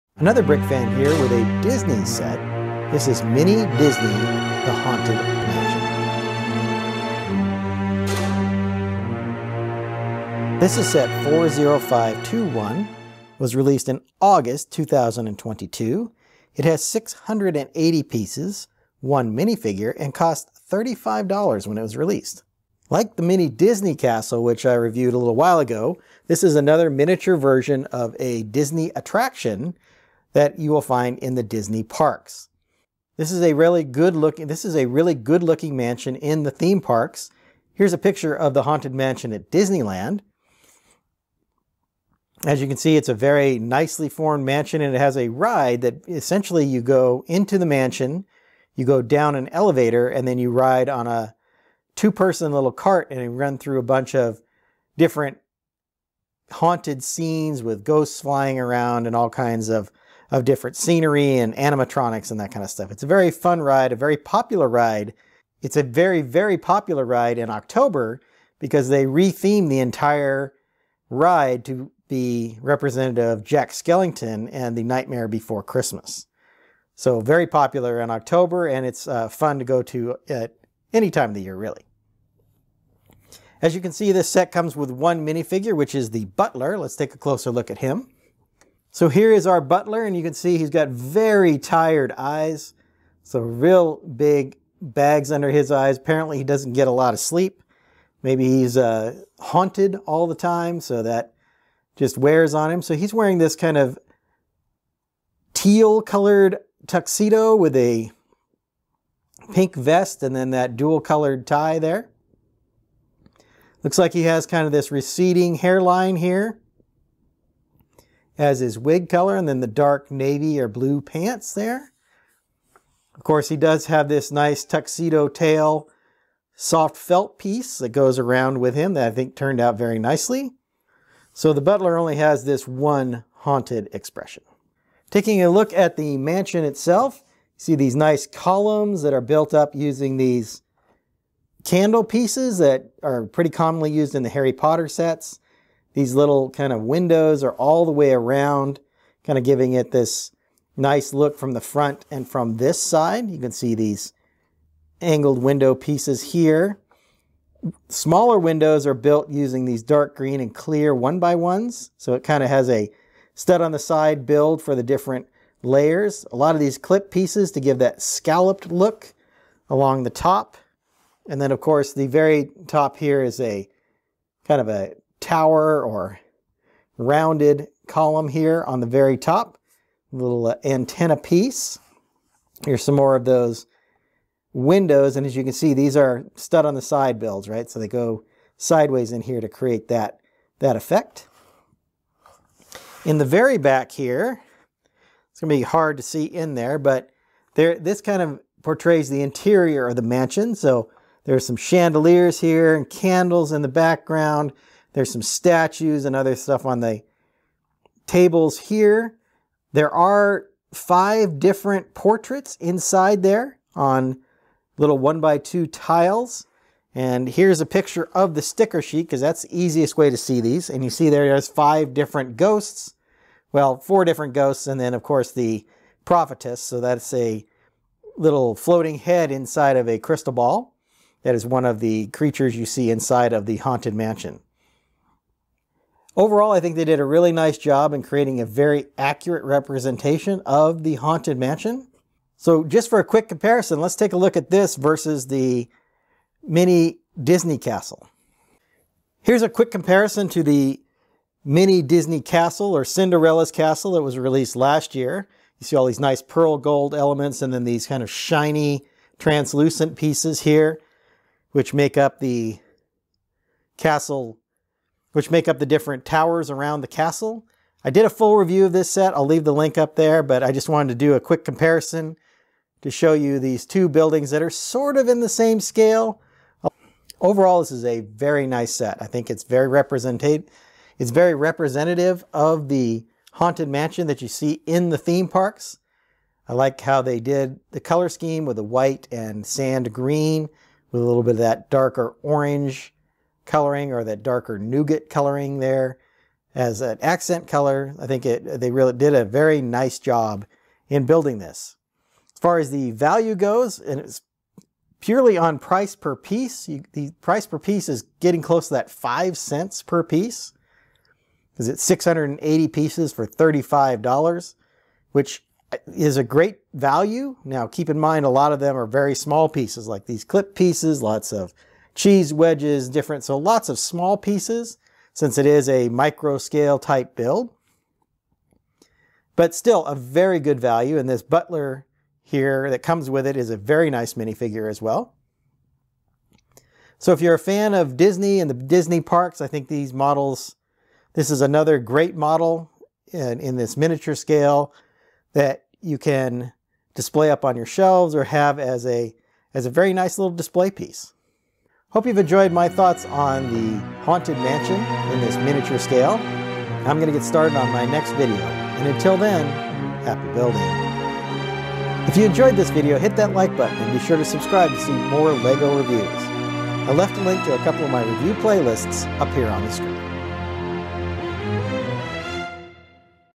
LEGO Mini Disney The Haunted Mansion review set 40521
LEGO Mini Disney The Haunted Mansion review. This is set 40521 was released in August 2022.

I get most of my set information from LEGO as well as Brickset

Join me on my other Social Media channels.